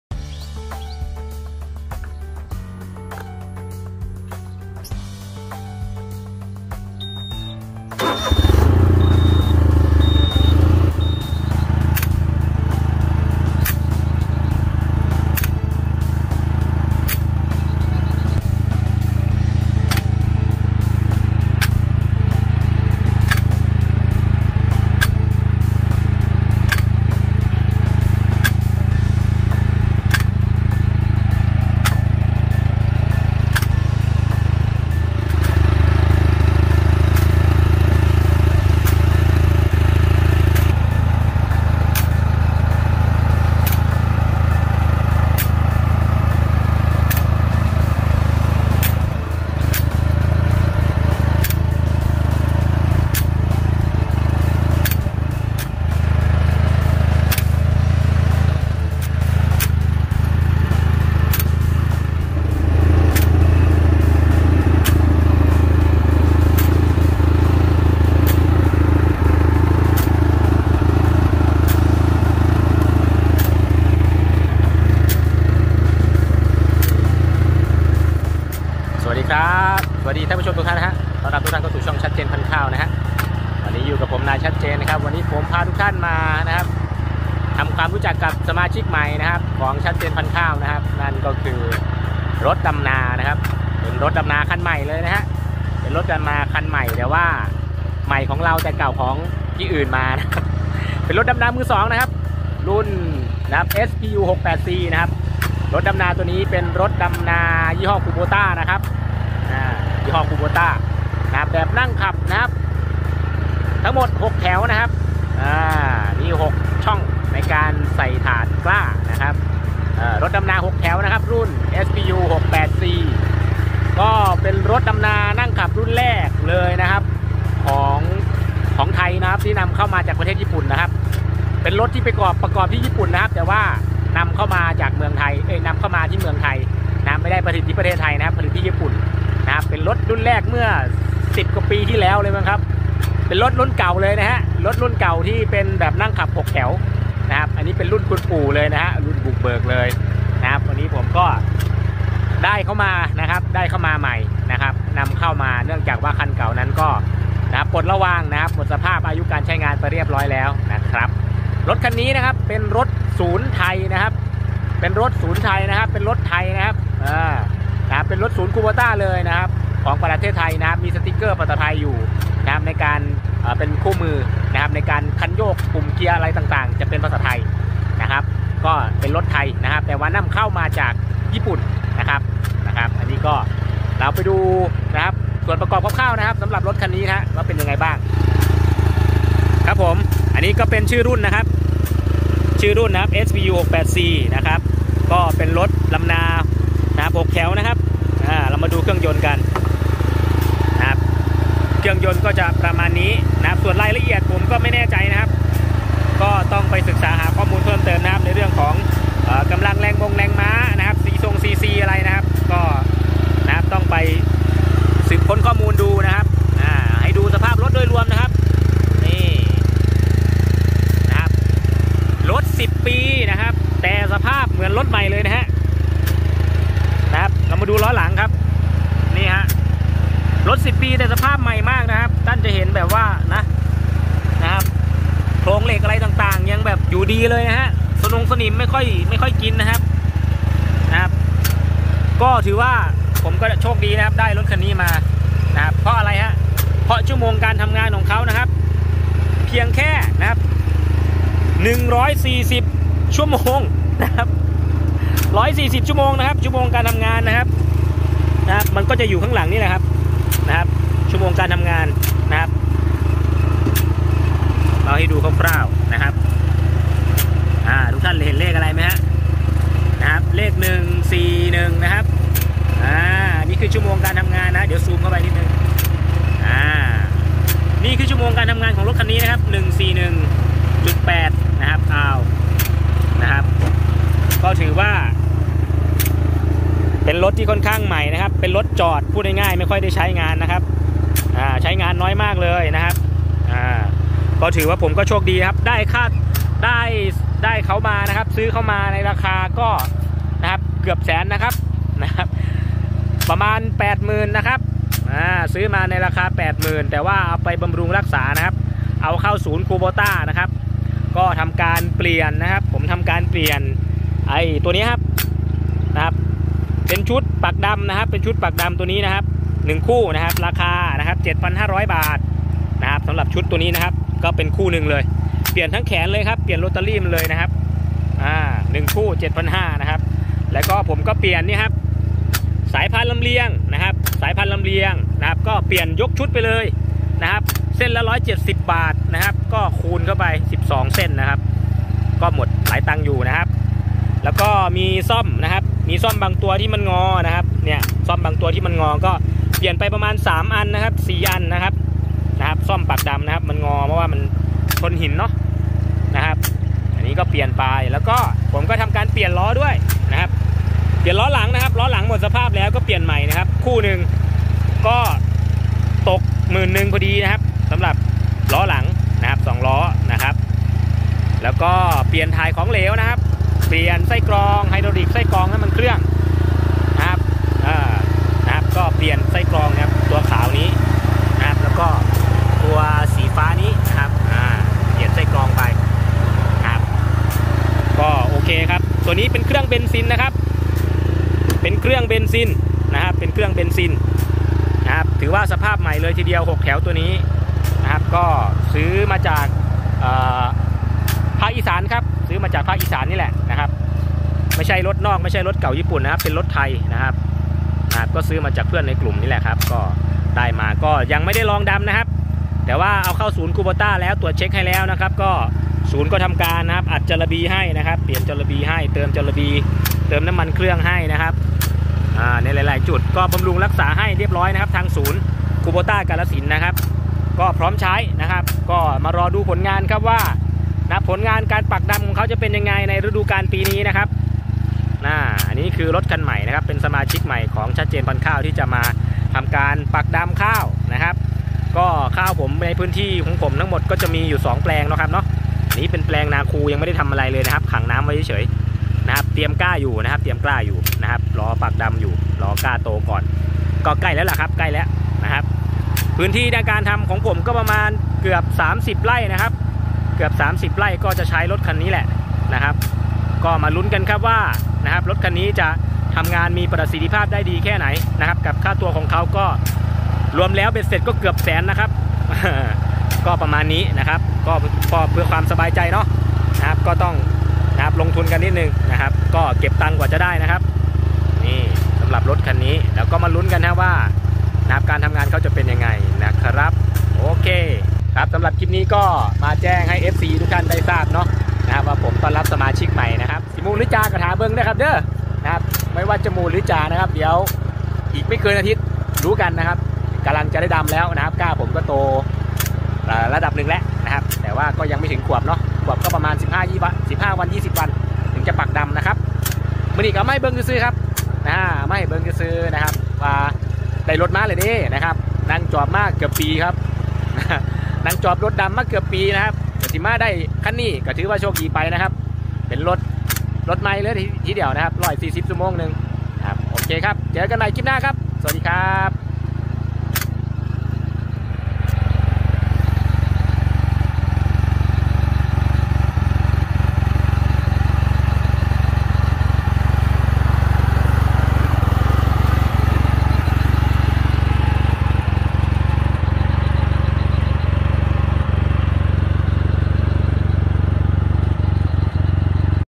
รถดำนานั่งขับ 6 แถว คูโบต้า spu 68c สมาชิกใหม่ ชัดเจนพันธุ์ข้าว
วันนี้ พาทุกท่านมาชมรถดำนานั่งขับ 6 แถว คูโบต้า spu 68c สมาชิกใหม่ ชัดเจนพันธุ์ข้าว

เพื่อเป็นกำลังใจในการทำคลิปต่อไปด้วยนะคับ🙏
อยากให้นายชัดเจน ไปทำคลิปเกี่ยวกับข้าวแบบไหน สามารถคอมเม้นบอกได้นะคับ
📌ชัดเจนพันธุ์ข้าว ตั้งอยู่ บ้านนาคู หมู่ 1 ต.นาคู อ.นาคู จ.กาฬสินธุ์
Facebook : เพจเฟสบุ้ค ชัดเจนพันธุ์ข้าว ตามลิงค์นี้เลยครับ =
📱LineID : janezudza